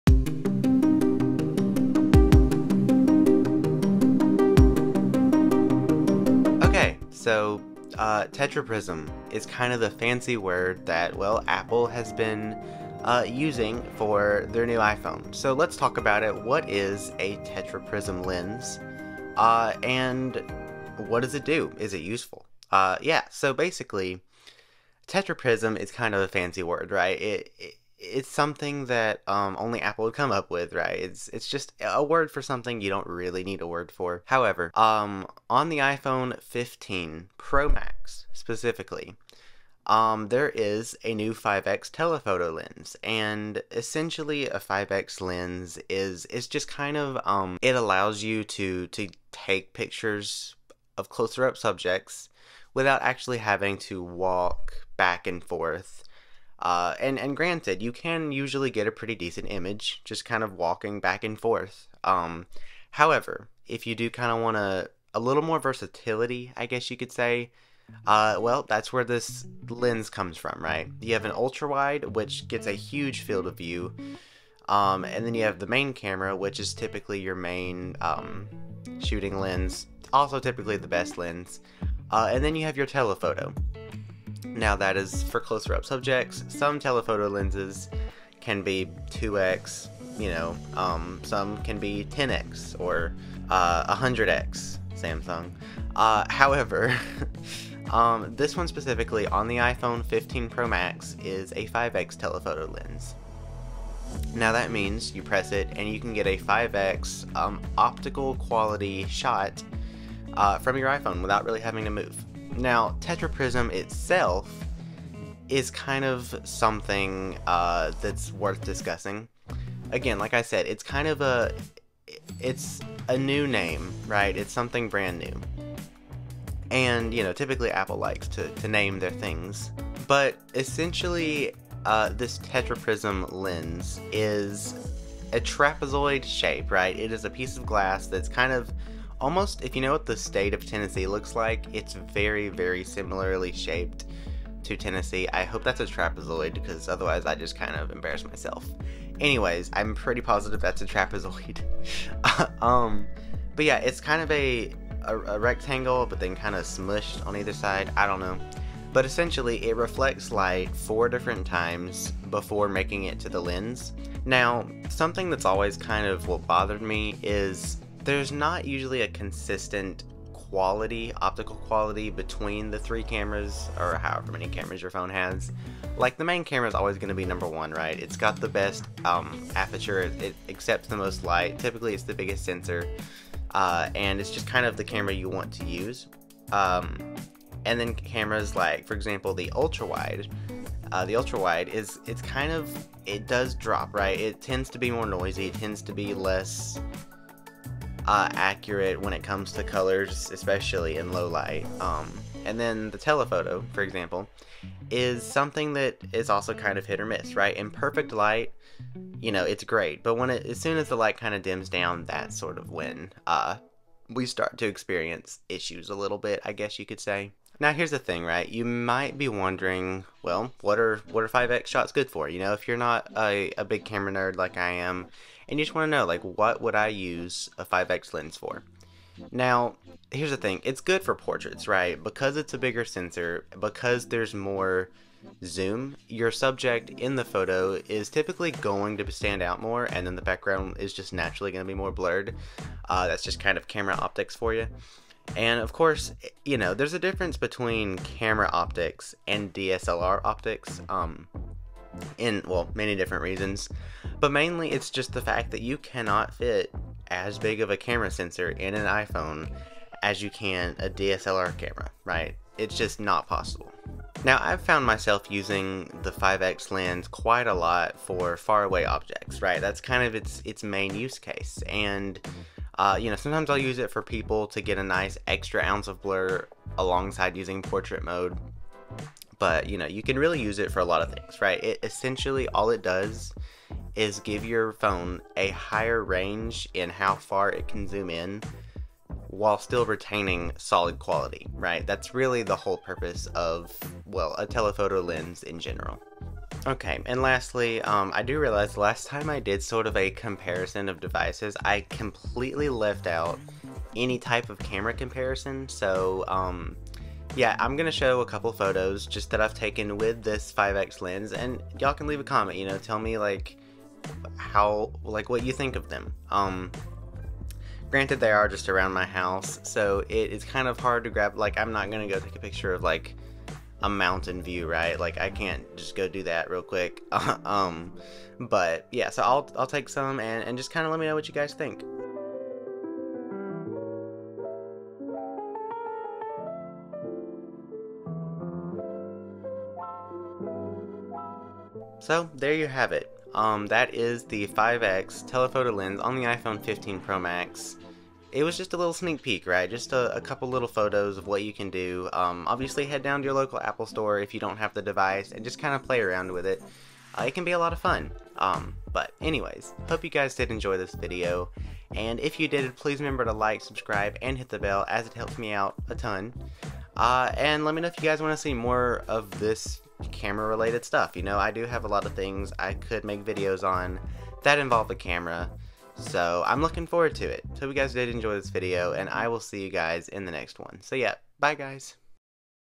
iPhone 15 Pro Max - 5x Tetraprism
In this video i talk about the iphone camera. I focus on the new 5x tetraprism lens, and I also display some test photos for comparison.

The tetraprism lens on the inside of the iPhone shot was created by Apple, go to apple.com to learn more about it! ￼

￼apple iphone15promax camera shotoniphone lens comparison review iphone tetraprism whatsinside software iPhone 15